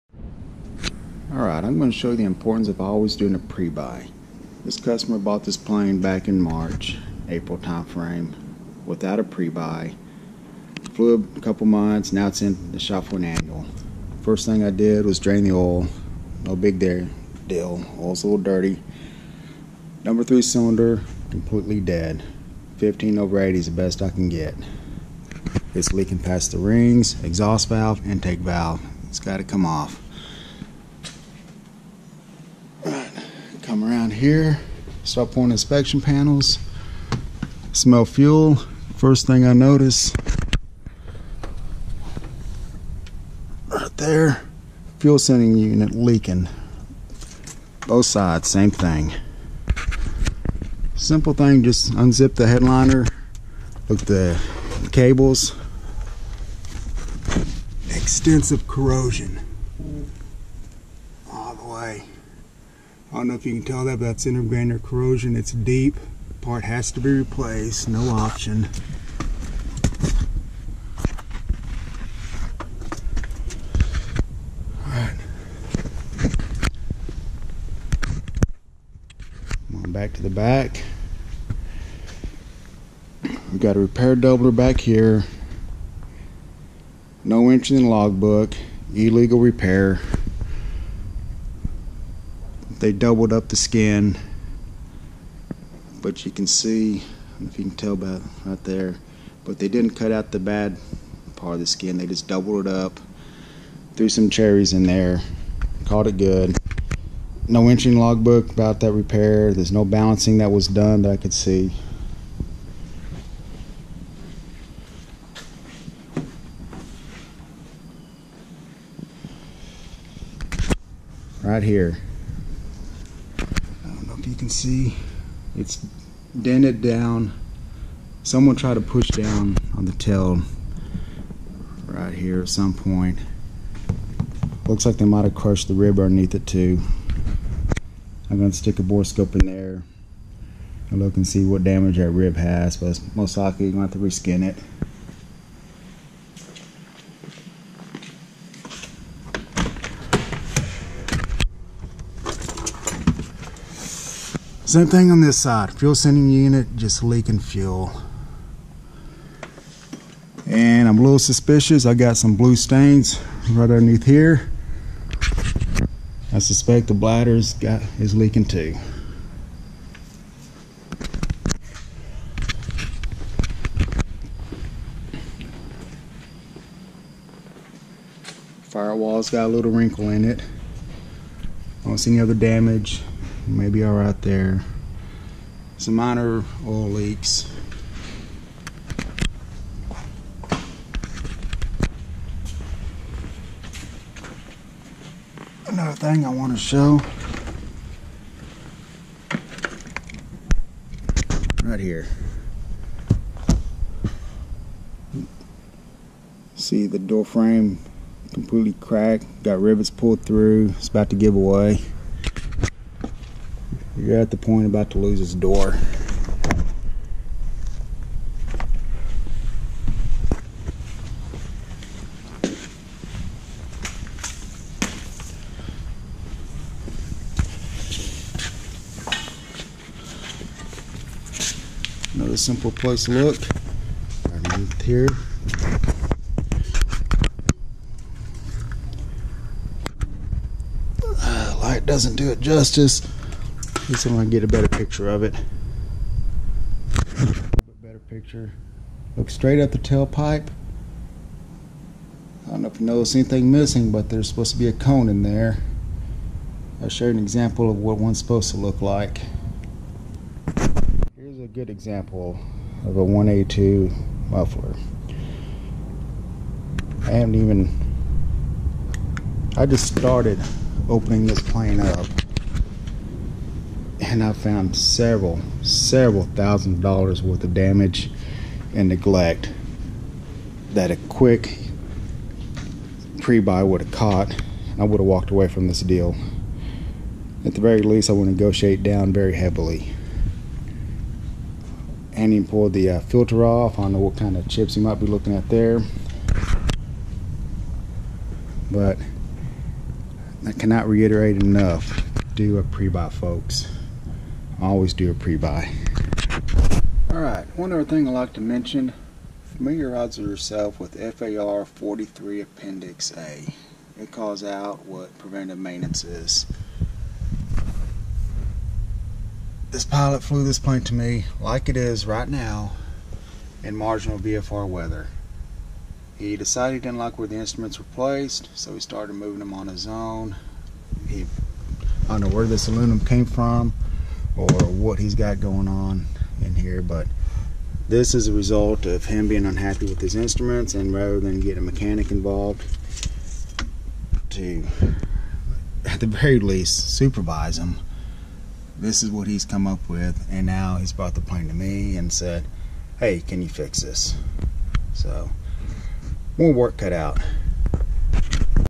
This is why you get a prebuy (wait till you see the instrument panel 😮)
Annual inspection went south for a green owner